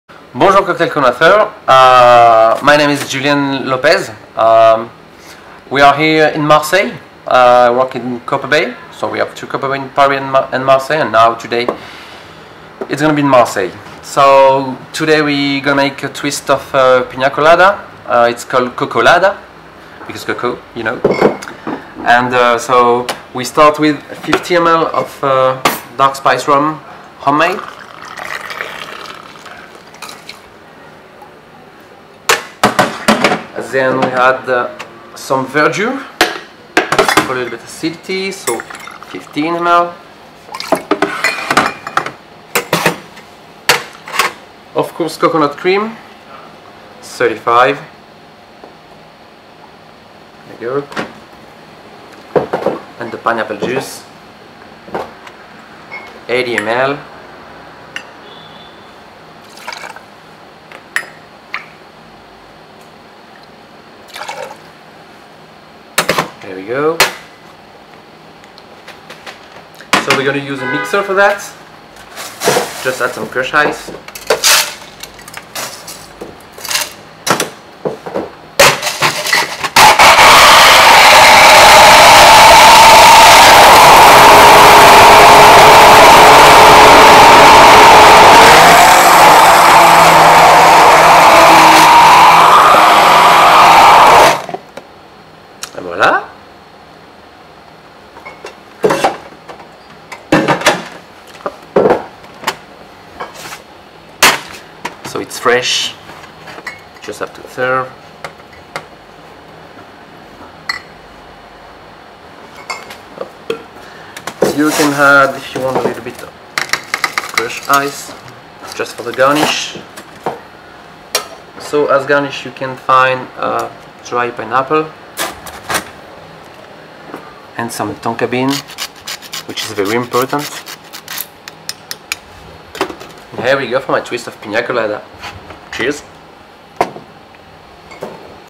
Julien Lopez from Copperbay (Marseille): Coco Lada
Julien Lopez from Copperbay made his twist on a classic Pina Colada cocktail for us, the Coco Lada